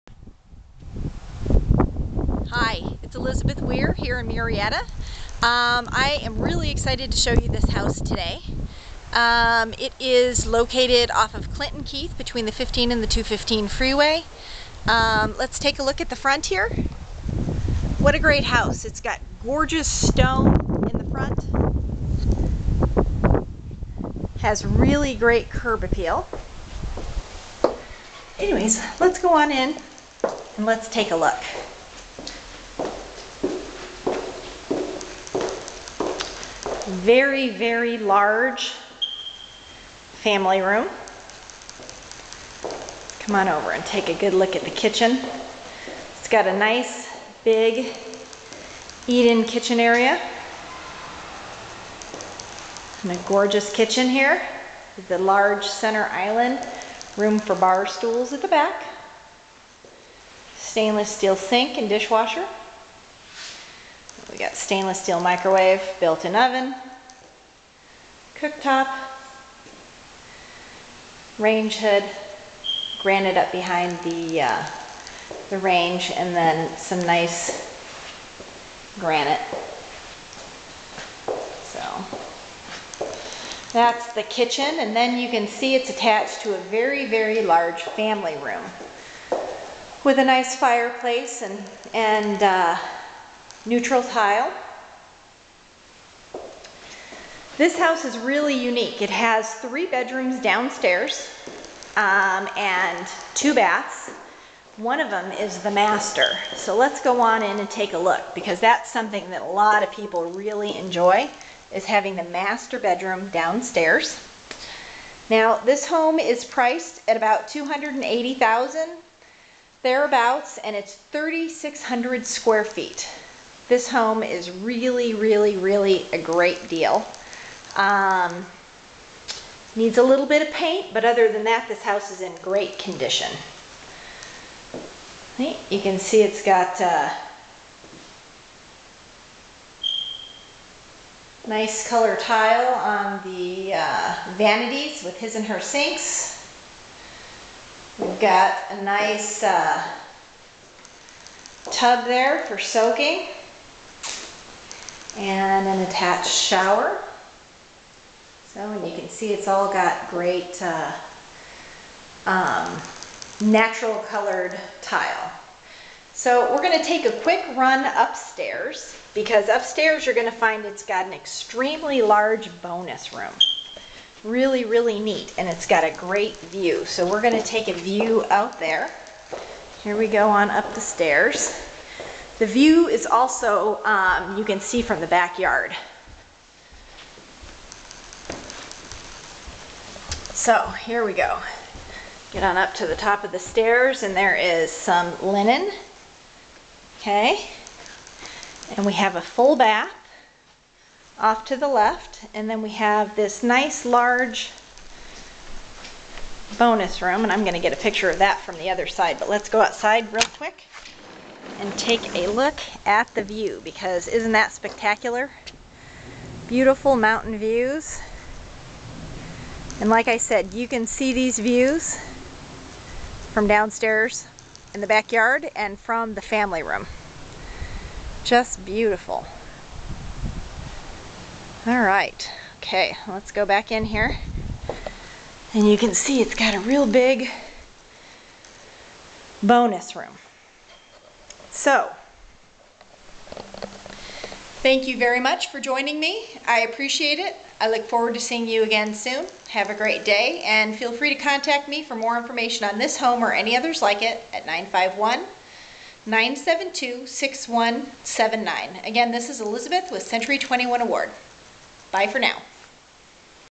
Murrieta View Home.avi
Wow!  What a deal!  This Bank Owned Home is more than 3500 sq. ft. and listed around $280,000!  This home is in great condition, all it needs is a little paint!.  The kitchen is very large with an island and plenty of room for informal dinning.  Granite counters and stainless steel appliances, tile floors and views, views, views!  Don't miss out on this one, this home also boasts a downstairs master suite, a 3 car tandem garage, indoor laundry and a bonus room with a deck upstairs.  Elizabeth Wehr, Century 21 Award.